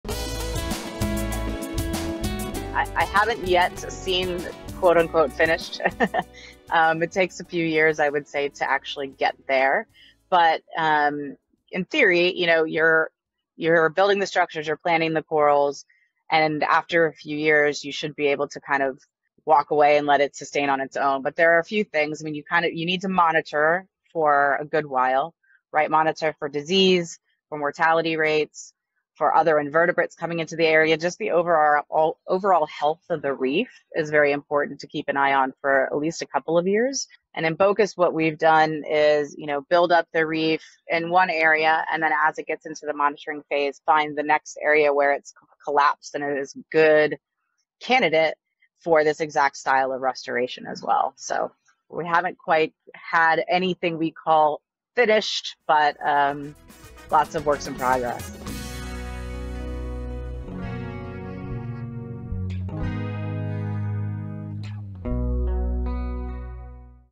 Krista Shoe: How do you know your coral work is finished?
Mother of Corals founder, Krista Shoe, explains her coral restoration work in Bocas del Toro, Panama, is almost a permanent work in progress.

Krista founded Mother of Corals in Bocas del Toro. Visit her site at: